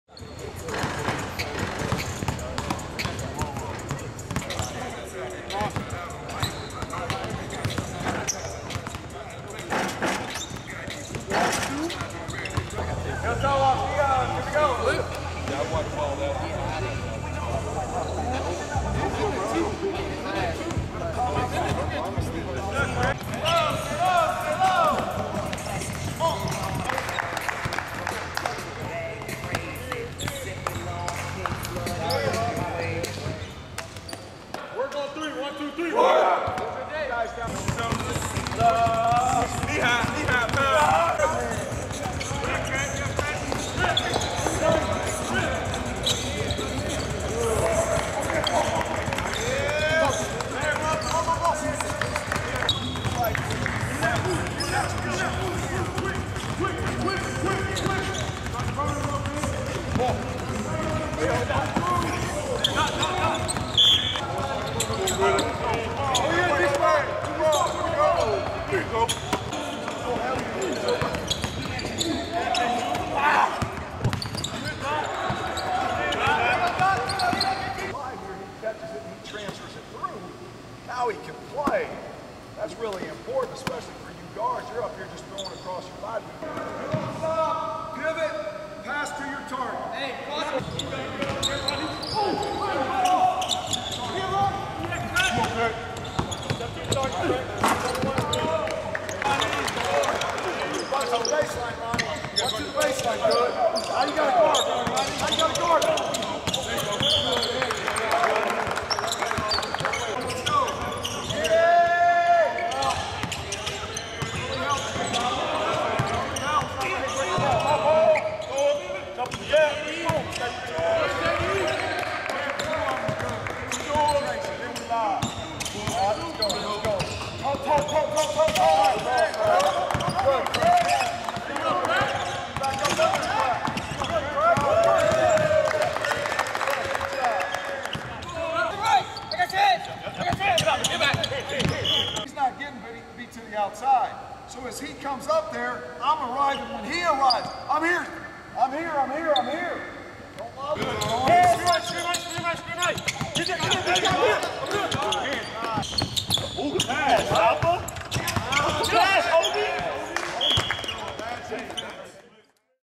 Sights & Sounds: MSU Men's Basketball
Check out some sights and sounds from the Bears' first practice of the season!